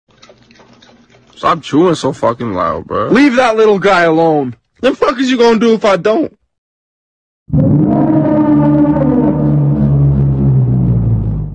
stop chewing so loud
He indeed chewed loud.

app used is sfm
idk how to do lighting

sound is from memes idk yeah probably